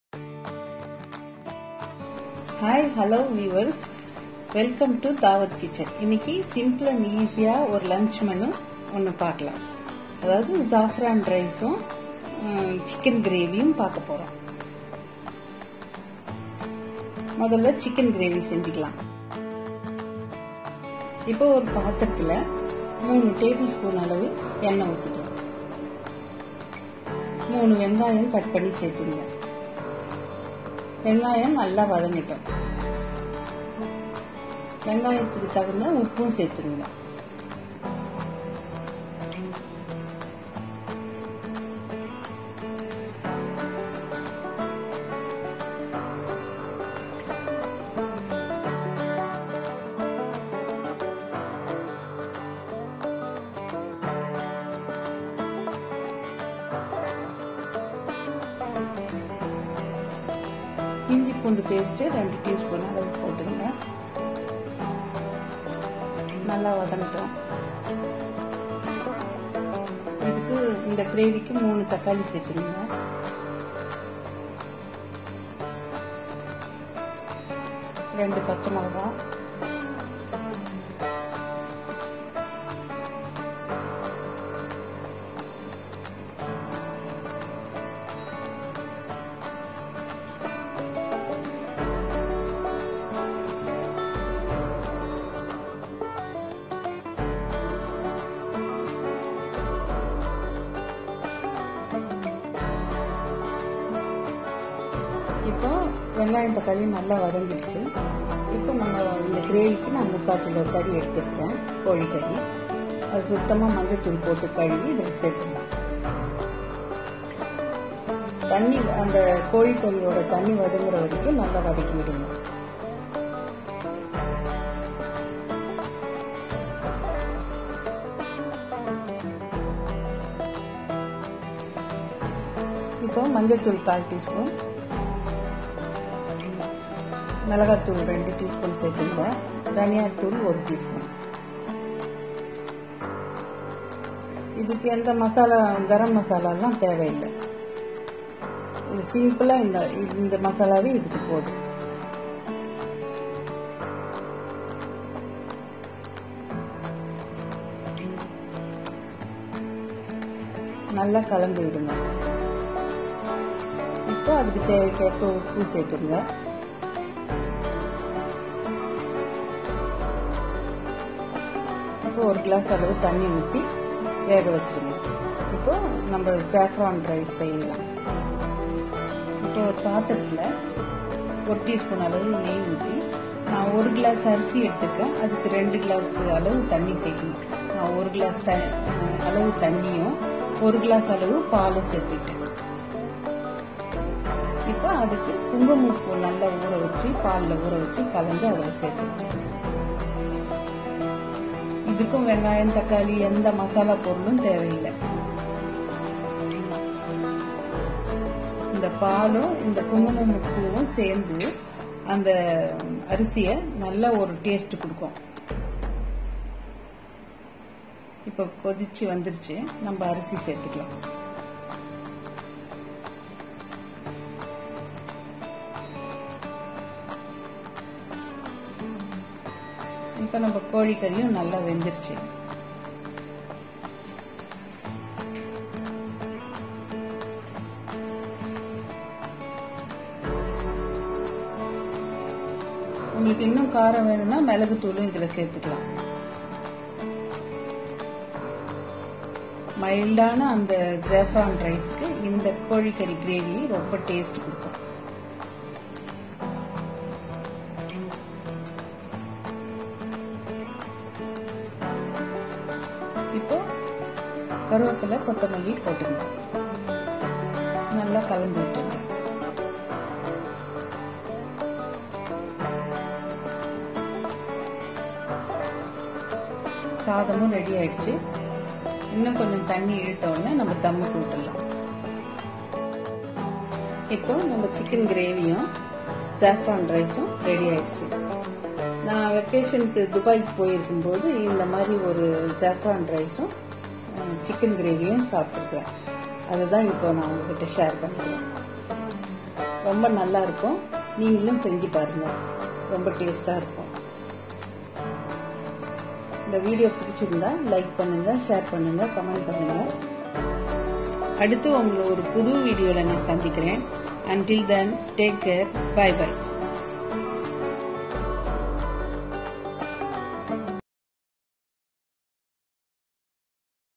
tasty tasty saffron rice with chicken gravy |lunch recipes| dawats kitchen
hi everyone asalamalaikum hope you all are safe. during my vacation to Dubai I had tasted saffron rice with chicken gravy in a restaurant. I like that taste so much and tried at my home many times it came successful and tasted as such...so shared this recipe with you all... please do try and enjoy this simple lunch recipe at your home...

watch other interesting recipe videos too